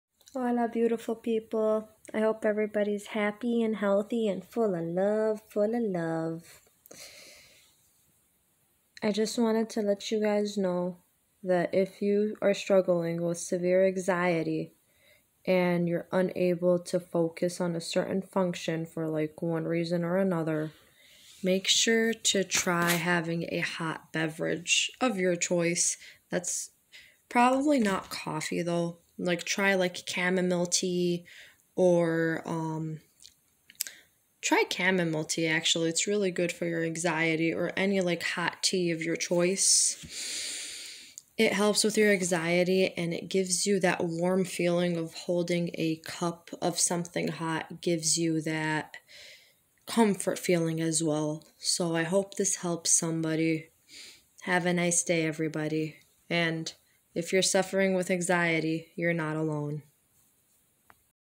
Hot Tea💖// Helps with Anxiety// You are not alone.
Hot tea can help with your anxiety. It gives you a feeling of warmth and comfort. It really does have a calming affect. If you are suffering from anxiety, you are not alone. I love you!💖